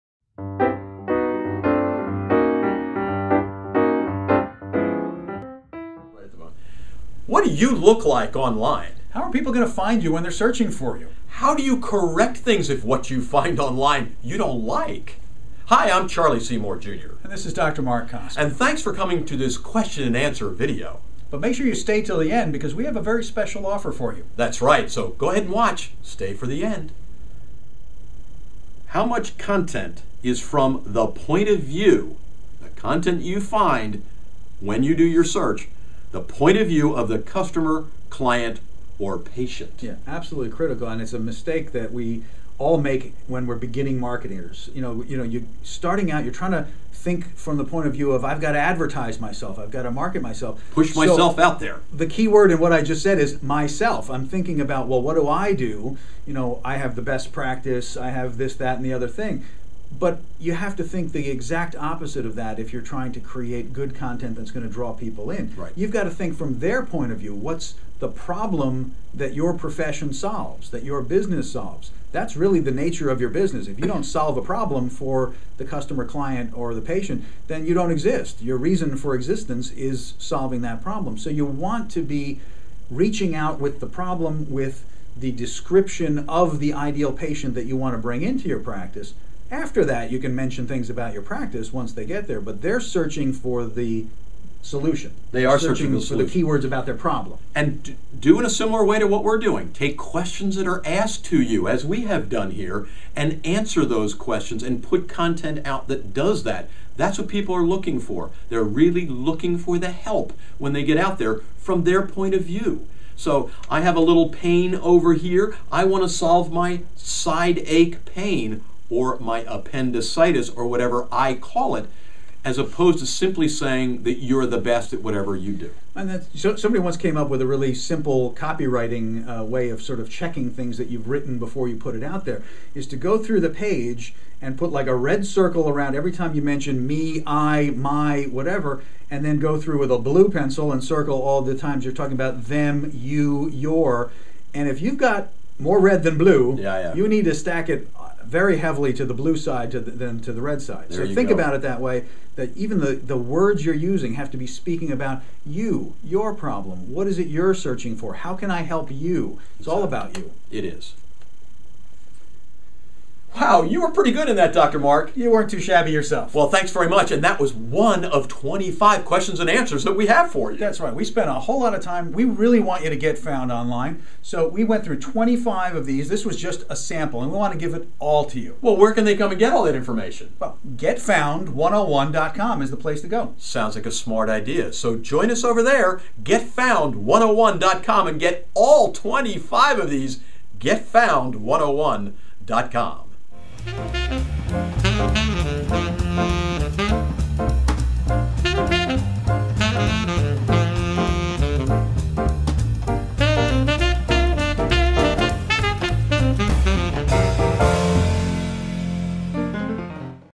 Client Relations - How Much Content Is From The POV Of Your Clients?
customer perceived value, patient's point of view, client's pov, patient's pov, Dr. Marc Kossmann, Charlie Seymour Jr, Dr. Marc and Charlie

When prospective clients, customers, or patients Google you before deciding to do business with you, what do they find?

What do you look like to the 80% of people who now check online before starting a relationship?

How do you set up and maintain a powerful online presence that draws in new clients and patients and makes existing ones raving fans and insanely loyal?

Watch this video to get the answer to ONE of the questions that Dr. Marc Kossmann and Charlie Seymour Jr discuss, and then click the link to get all 25 questions and answers free! Learn how YOU can improve your online profile, get more clients or patients and connect with your marketplace.